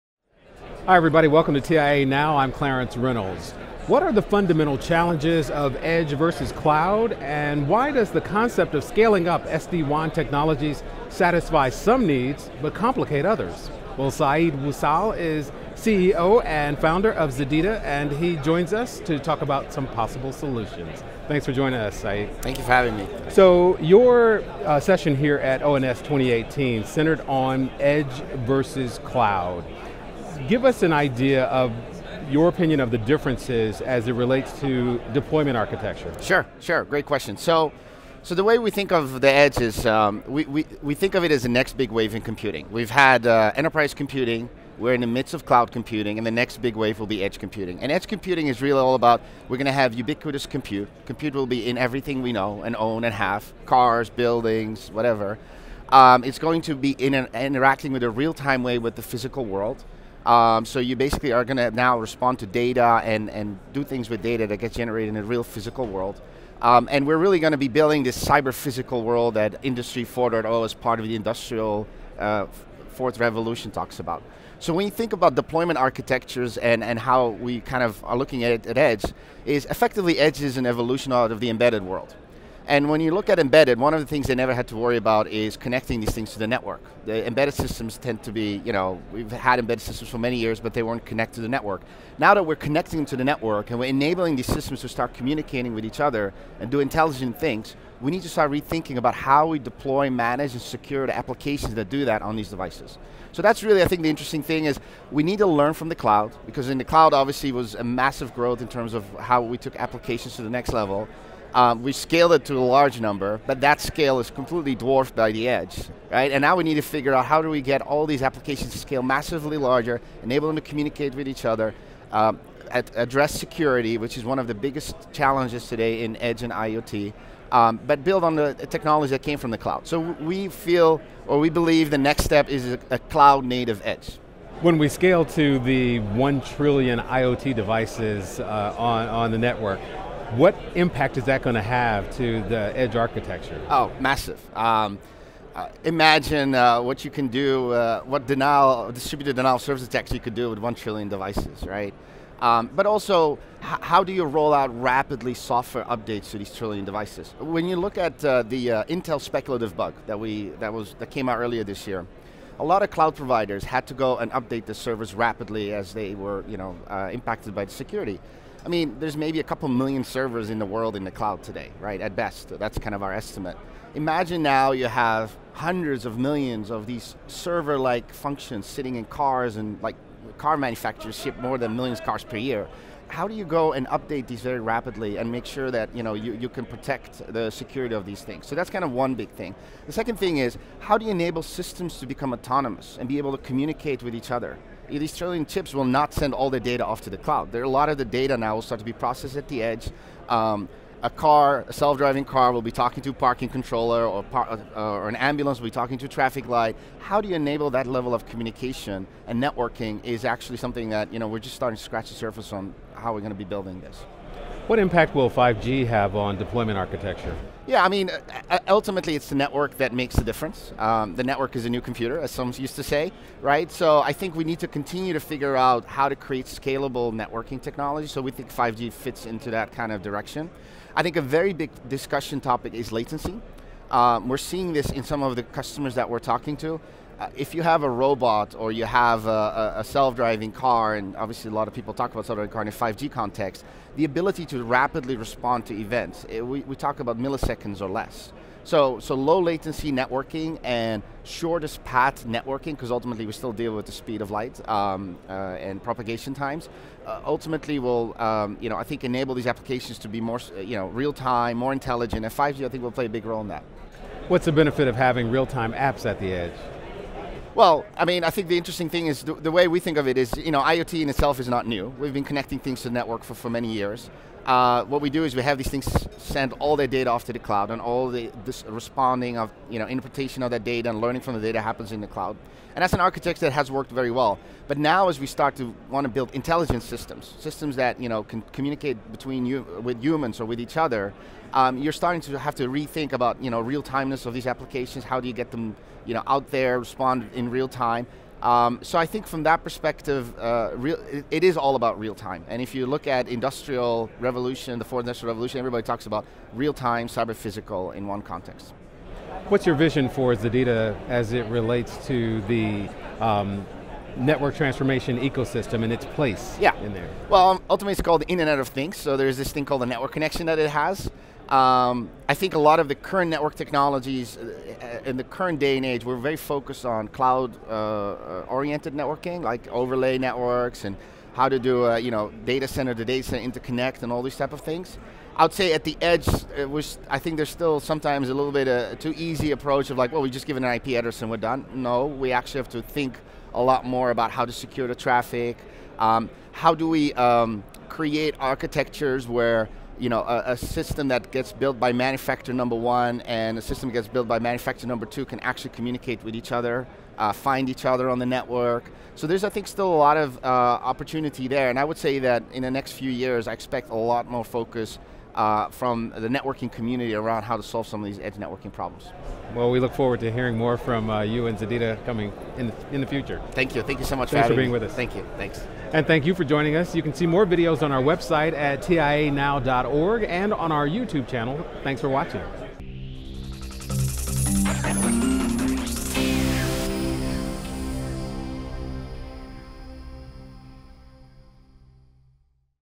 Brain Meets Brawn in the Edge vs. Cloud Debate
What are the fundamental challenges of edge computing vs. cloud computing? Are there benefits to having real-time apps at the edge? Said Ouissal, CEO and Founder of ZEDEDA, joins TIA NOW to discuss the mutually beneficial coexistence of both edge and cloud.